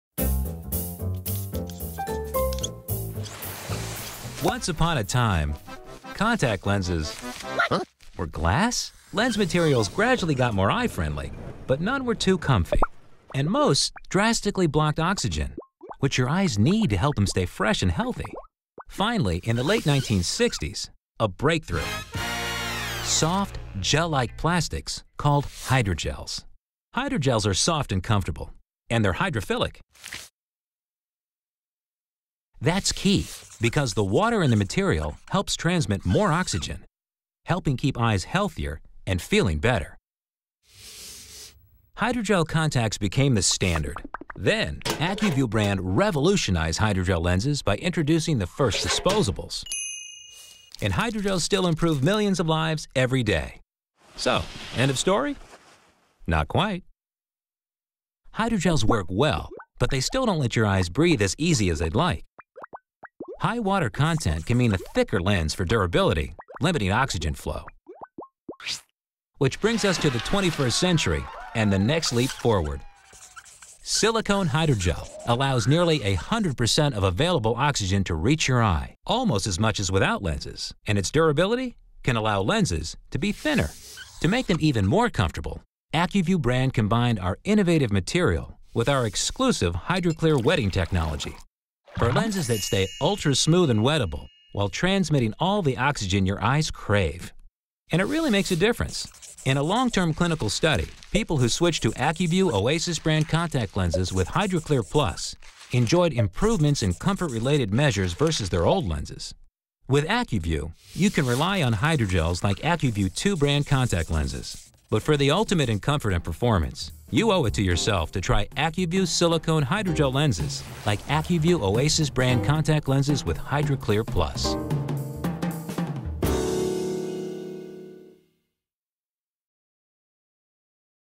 Silicone Hydrogel Material & Hydrogel Material
Would you believe the earliest contact lenses were actually made of glass? It's true. Come follow the story of how we came to enjoy the benefits of the latest soft, comfortable lenses made with Hydrogels and Silicone Hydrogels.